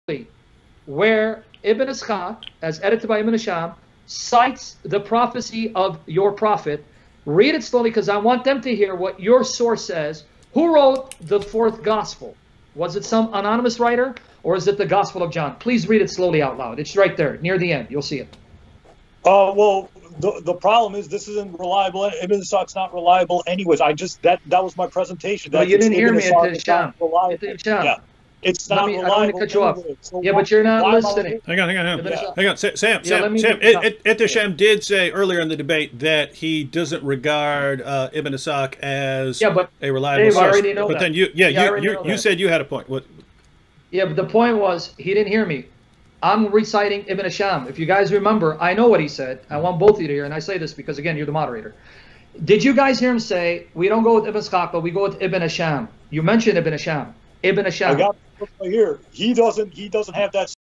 Sam Shamoun caught being dishonest on Ibn Ishaqs Reliablity By Ehteshaam Gulam
In the beginning of the debate I said why Ibn Ishaq is not reliable. Sam Shamoun tried to pull a fast one and say he was reciting Ibn Hisham (which is a little bit more reliable). However I had the Ibn Hisham book and knew he wasn't being honest. Beware of these lies that he, David Wood and the other critics of Islam say.